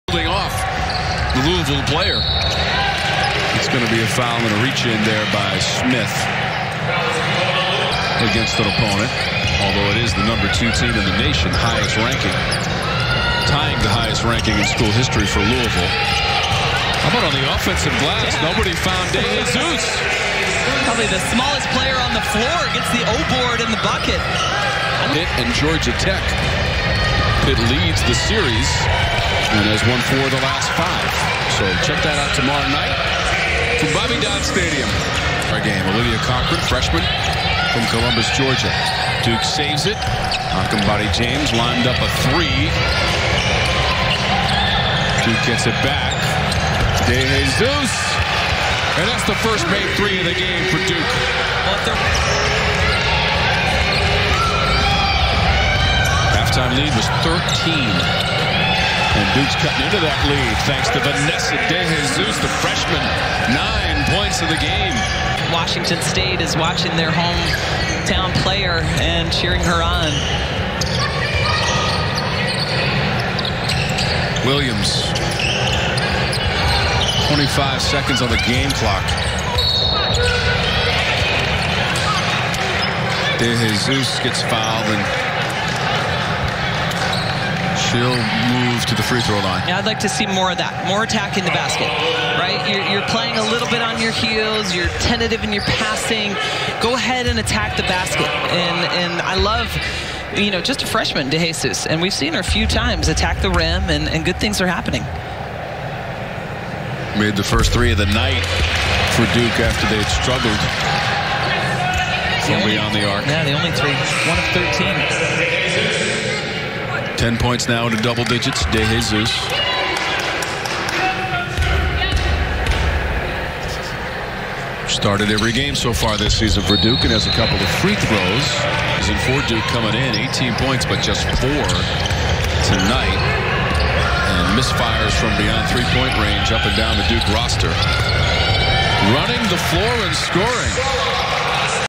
VANESSA DE JESUS 4TH GAME VS LOUISEVILLE / NCAA WOMEN'S BASKETBALL
In the battle to remain undefeated, the perennial contender Cardinals ultimately proved too strong for the upstart Blue Devils playing in their first conference game of the Kara Lawson era.

No. 2 Louisville bested Duke 73-49 Wednesday night in Cameron Indoor Stadium, handing Lawson her first loss as the Blue Devils’ head coach.